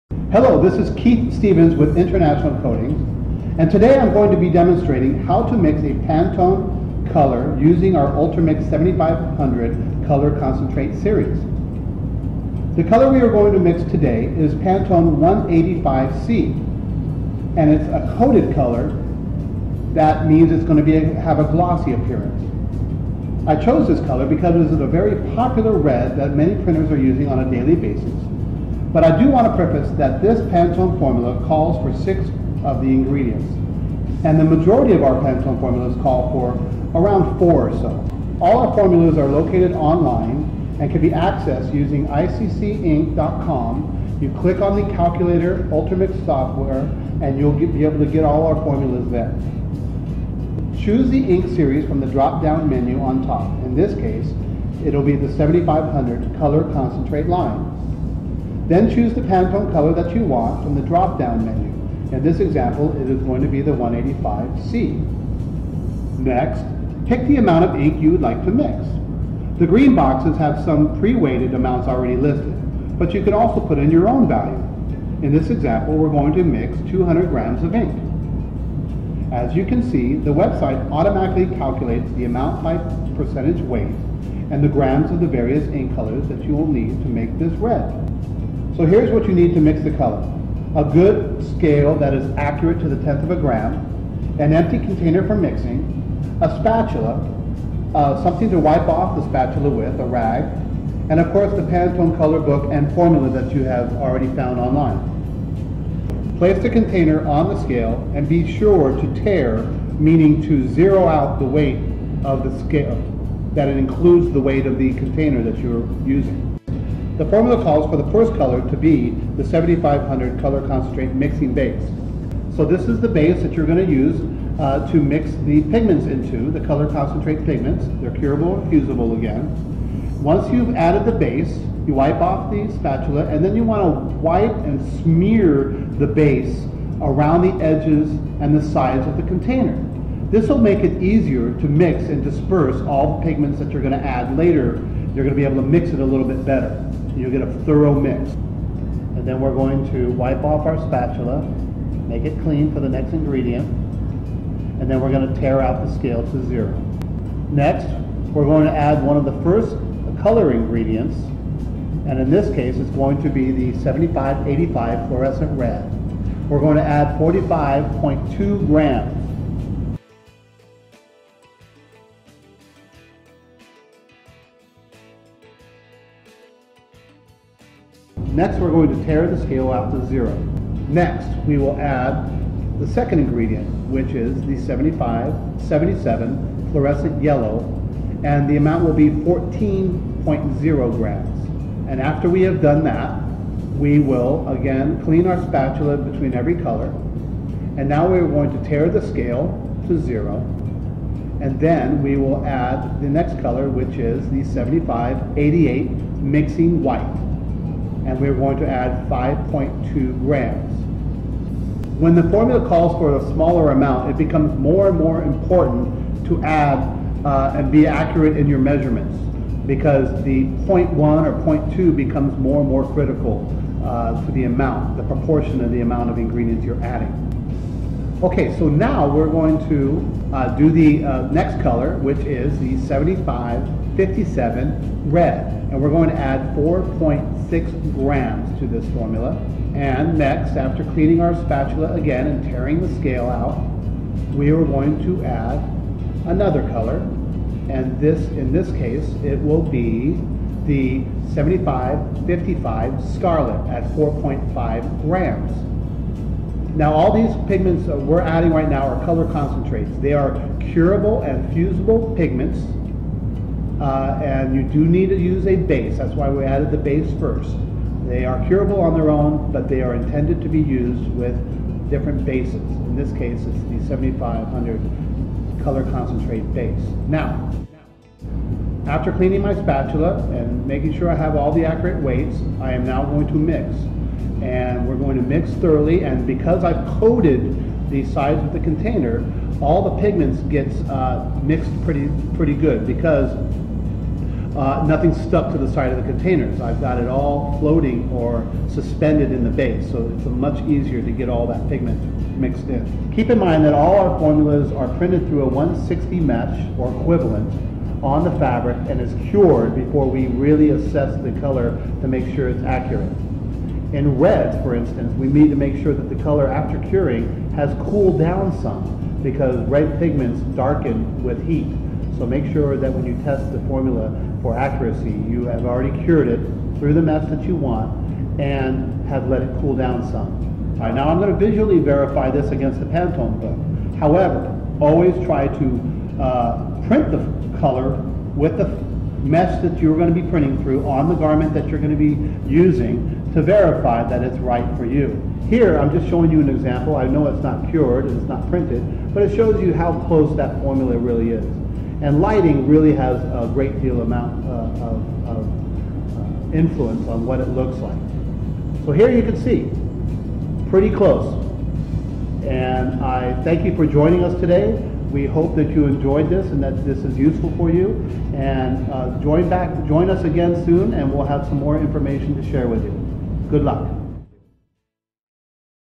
Match PMS Colors, 7500 Color Mixing System | International Coatings 7500 CC PMS
Your customers are demanding exact color matches for their logos or need multiple colors matched for a design? Not sure how it works or how complicated the process is?

Invest in a color matching system! International Coatings' UltraMix® 7500 Color Concentrate Mixing system allows you to quickly and economically mix and match Pantone® colors.

This video showcases how to easily mix and match Pantone® colors using the UltraMix® 7500 Color Concentrate color mixing system for screen printing inks. The 7500 Color Concentrate system is very versatile as the color can be mixed into a variety of bases. For example for an athletic or performance fabric, you can use our Performance-Pro 7100 base and change it into any color. Similarly it can be used to tint the soft-hand base, HD base, Matte base, Nylon base, or the Very Opaque Mixing base.

You can also determine the quantity you would like to mix - from a pint to multiple gallons, International Coatings' online formulation calculator can do it all.

Squeegee and Ink - a company located in the UK - also did a great tutorial.